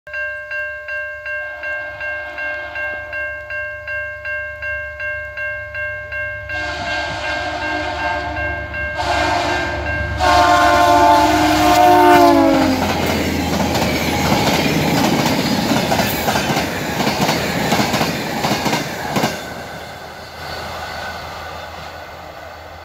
(J-Train) VIA 75 - 6435 And VIA 82 - 919 With Budd Cars In Lynden Ontario
It's cool to see a J-Train again but I'm getting really annoyed with seeing 919 with it's budd cars. this is the 5th sighting of that assignment in just about a week.